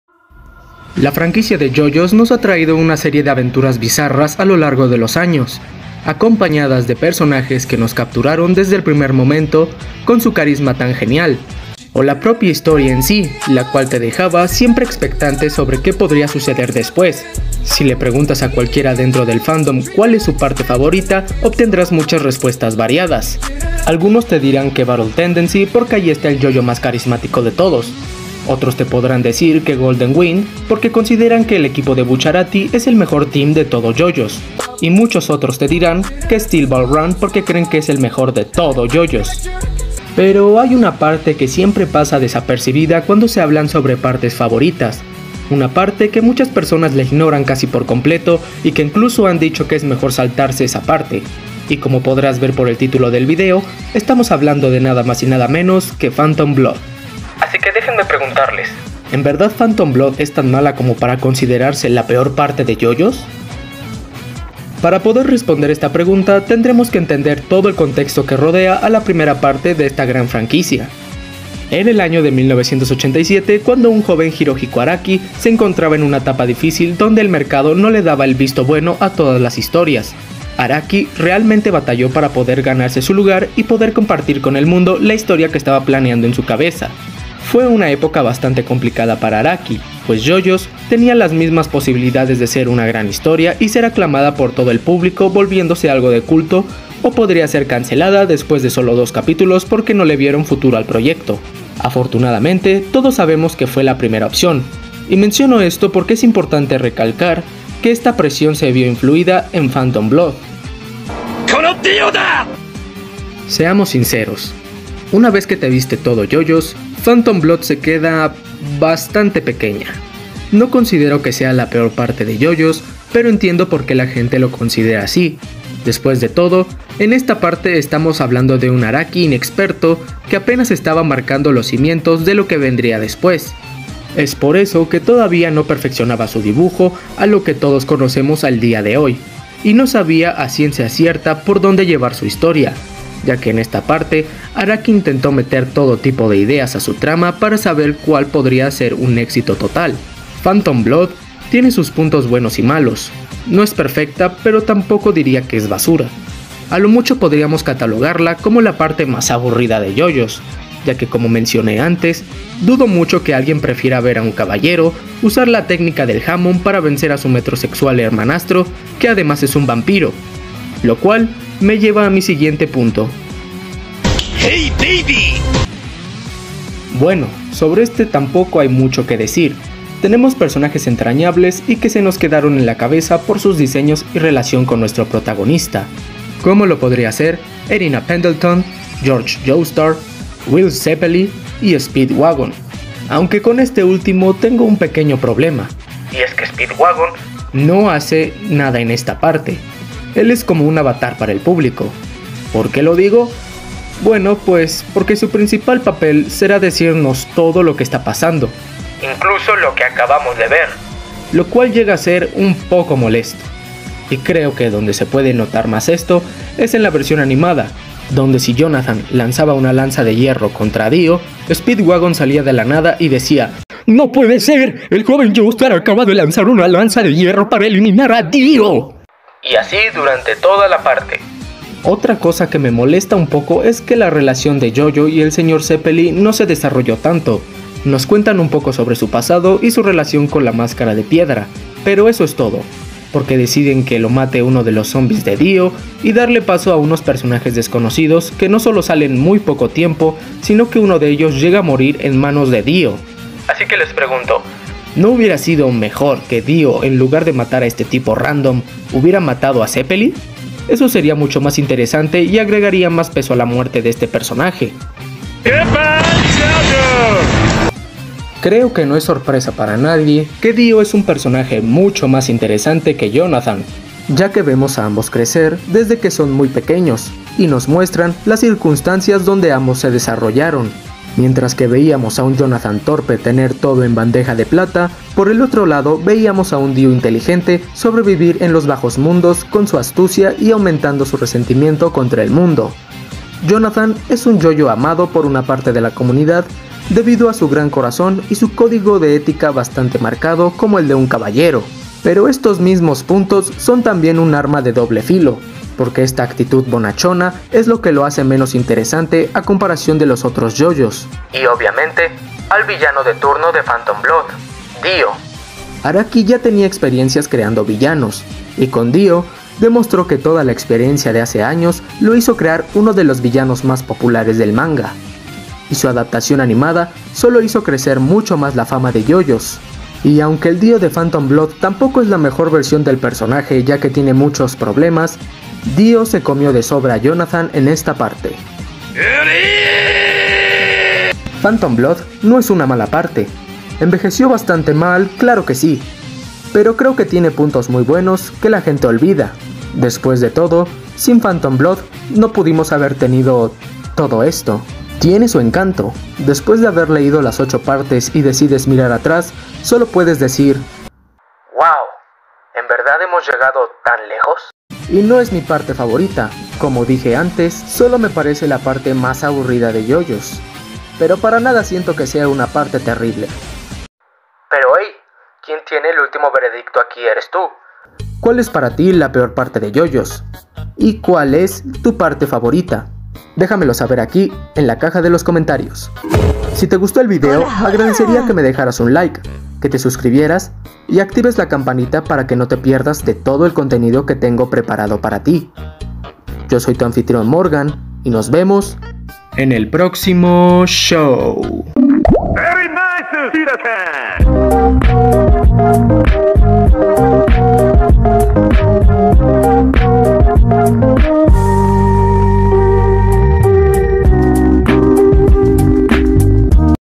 ¿PHANTOM BLOOD es la PEOR PARTE de TODO JOJO'S? || RESHOWSPECTIVA
Hola a todos! En este video daremos una pequeña "Reshows´pectiva" sobre Phantom Blood, la cual es considerada por muchos como la peor parte de Jojo´s

Un agradecimiento especial a mi amigo Rinov por la hermosa canción final de estos videos, dale un vistazo a sus redes para no perderte ninguna de sus fabulosas canciones, no te arrepentirás ;)